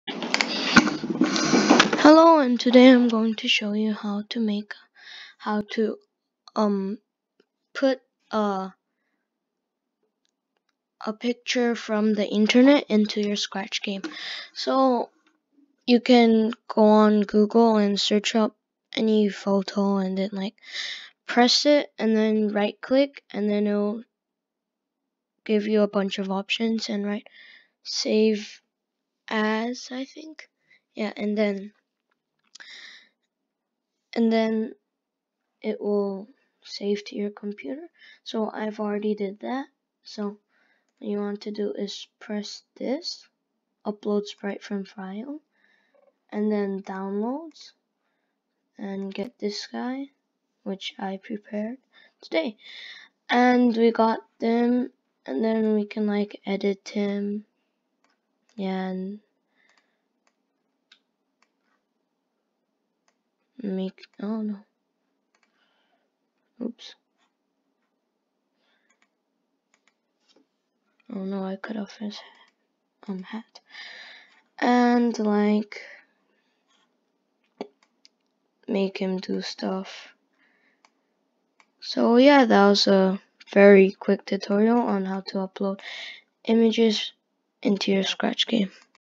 how to put pictures from google into your scratch game
in this very simple and quick tutorial, i show you how to easily put pictures from google into your scratch game.